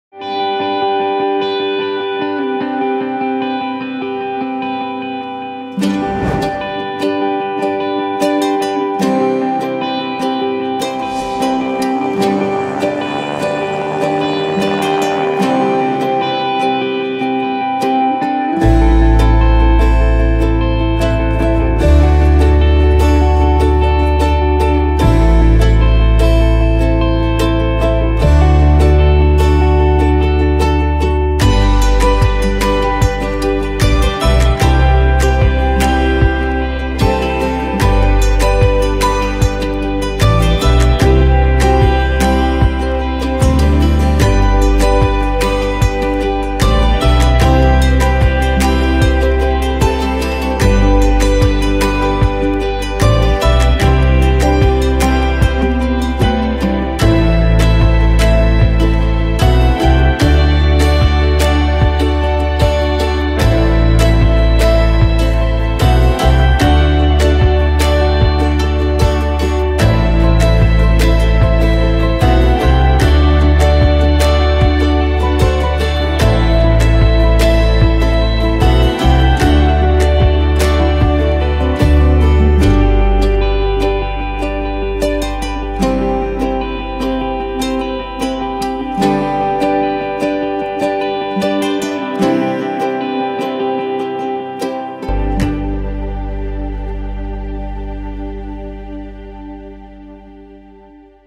MSC World Cruise 2020: Papeete - Wellington with The World of Sisters
Follow Cindy from The World Of Sisters as she explores the wonders of the Pacific for the MSCWorldCruise 2020 on board MSCMagnifica.